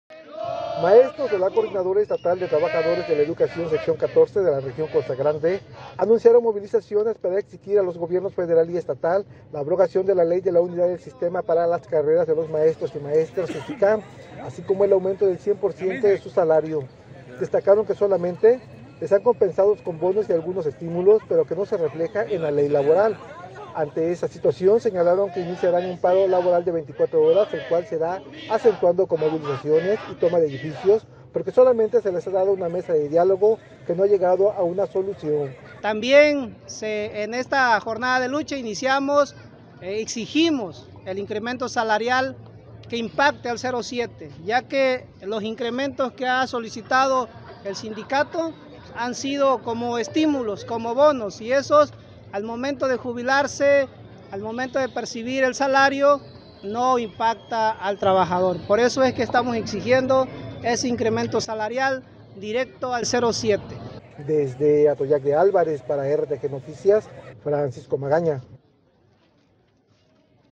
RTGnoticias | Anuncia movilizaciones CETEG en la Costa Grande
Maestros de la coordinadora Estatal de trabajadores de la educación sección 14 de la región Costa Grande, anunciaron movilizaciones para exigir a los  Gobiernos Federal y Estatal la abrogación de la ley de Unidad del Sistema para la Carrera de las Maestras y Maestros Usicamm, así como el aumento del cien por ciento de su salario.